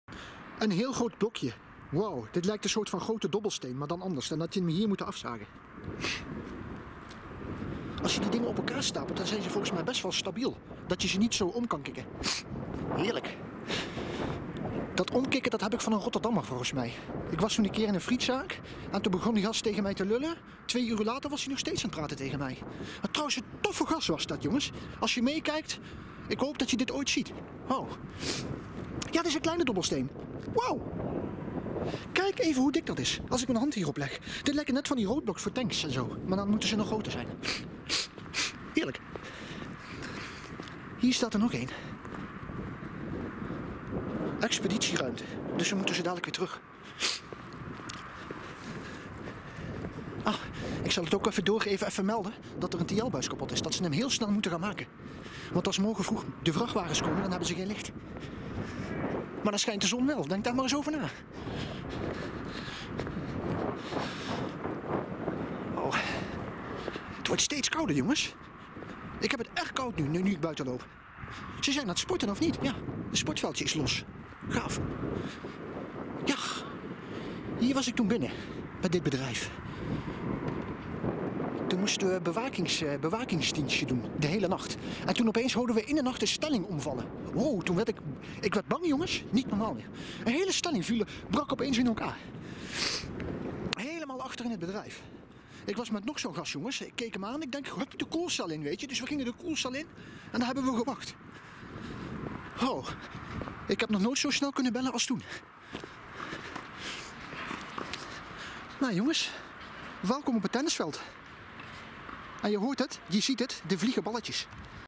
Dobbelsteen van 1000 Den Kilo, s Zwaar
Deze til je niet Zomaar op ook niet denk ik dat je de Sterkste Man van Nederland Bent